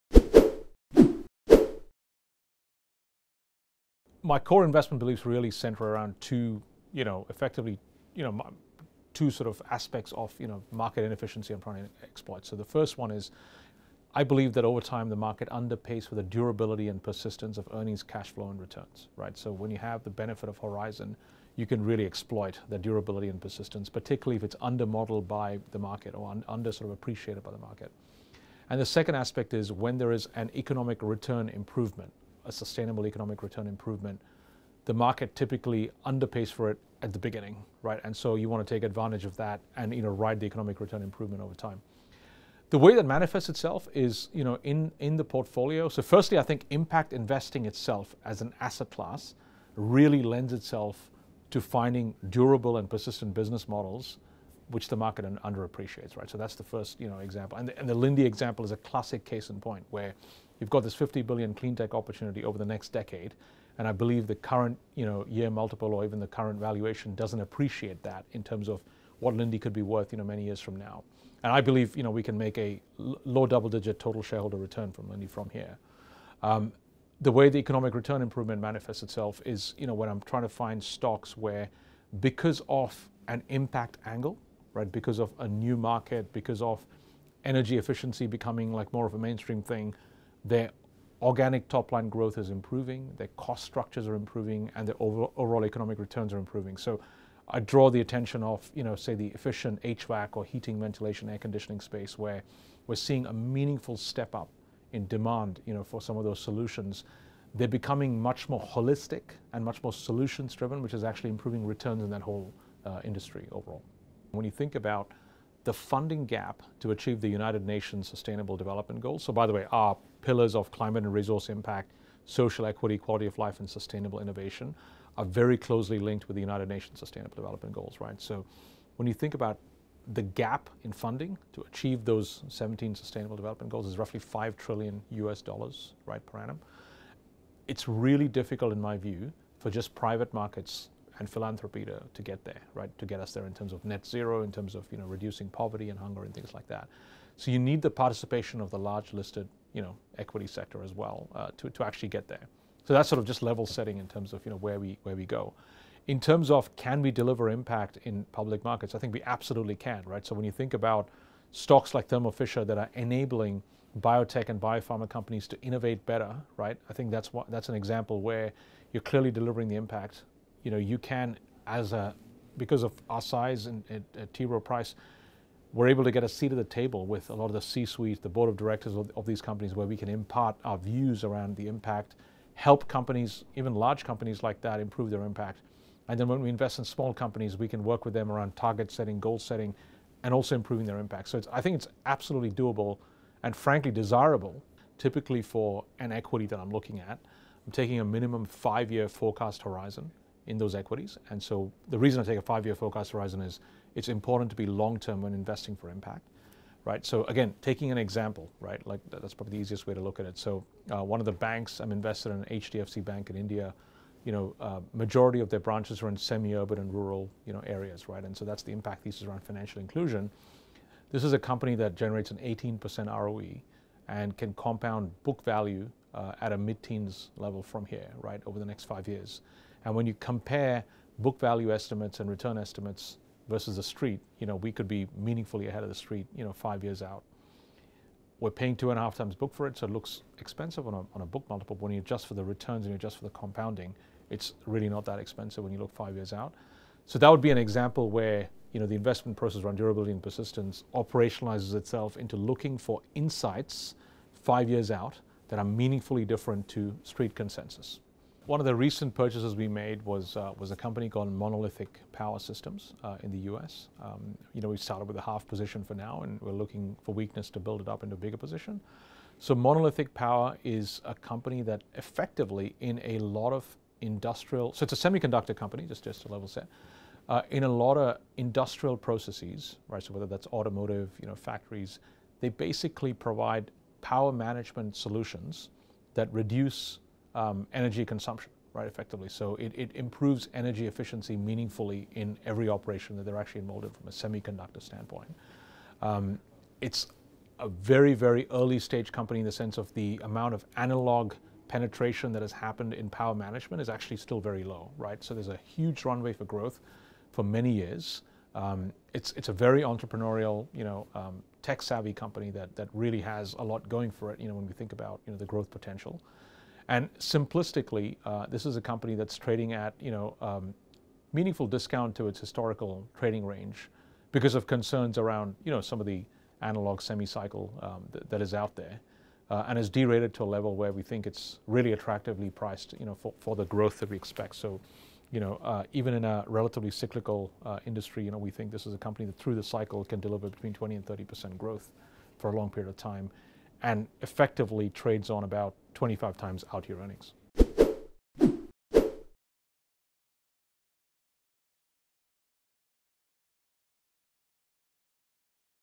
How T. Rowe Price generates impactful alpha (and two stocks to prove it)
At the fund's big picture level, Balkrishna and the team focus on companies that can generate a quantifiable footprint around at least one of three key themes:

- Climate and resources
- Sustainable innovation and productivity
- Social equity and quality of life

But he is also very quick to point out that just because he runs an impact fund does not mean he is willing to sacrifice all returns for the feel-good factor. As he puts it, "this isn't charity."

So how do you create a process that balances impact and earnings appreciation? He says it's all about finding durability - companies which can be bought at an attractive valuation, produce quality earnings for five years or more, and can leave a durable change.

In this edition of Expert Insights, Balkrishna gives us an example of a stock that can do exactly that. Plus, he shares details about a recent inclusion into the fund.